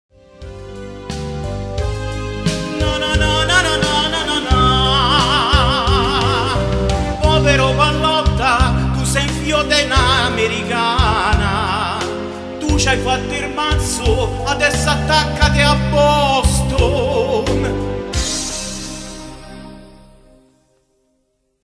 Roma - lazio 3-0: povero Pallotta tu sei...
sappiamo il momento di dolore che ha colpito anche il povero Pallotta dopo il 3-0 del derby di ieri...

un saluto anche a lui...